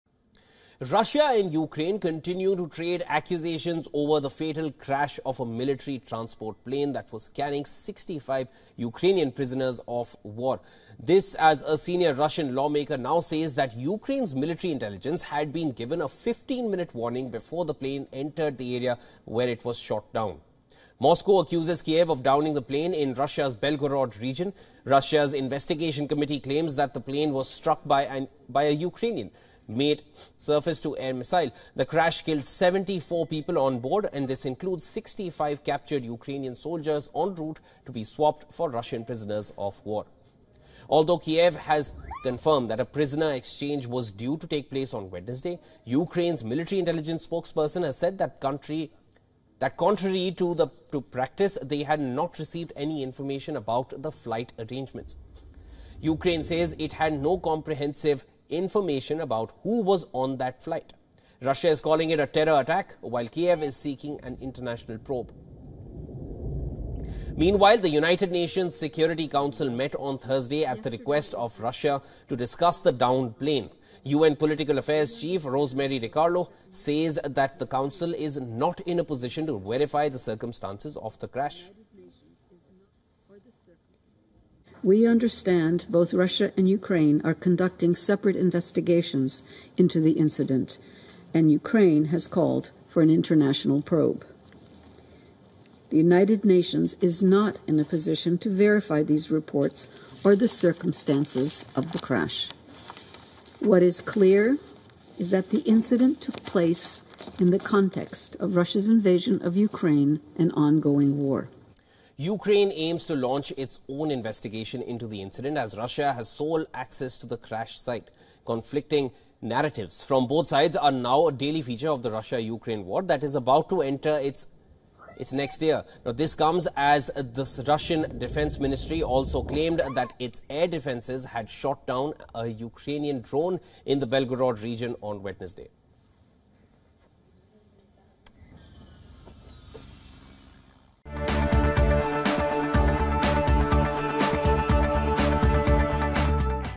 Mysterious Russian plane crash raises more questions than answers | WION
More than 24 hours since a Russian military jet crashed. Moscow says Ukraine had shot down the plane and killed dozens of its own soldiers ahead of a planned prisoner exchange.